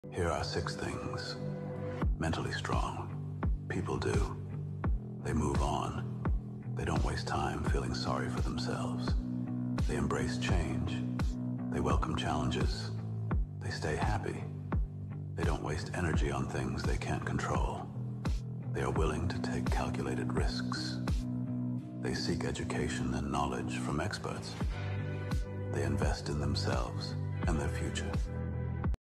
Secrets to unbreakable mental strength in just seconds! 💥
🔑 🔓 the secrets of mental fortitude with these six empowering habits!

💪 From gracefully moving on from setbacks to embracing change with open arms, mentally strong individuals know how to navigate life's challenges with resilience and determination.

They prioritize happiness, focus their energy on what they can control, and aren't afraid to take calculated risks for personal growth.

Ready to unleash your inner strength? Start today and embrace a life filled with courage, resilience, and unstoppable positivity!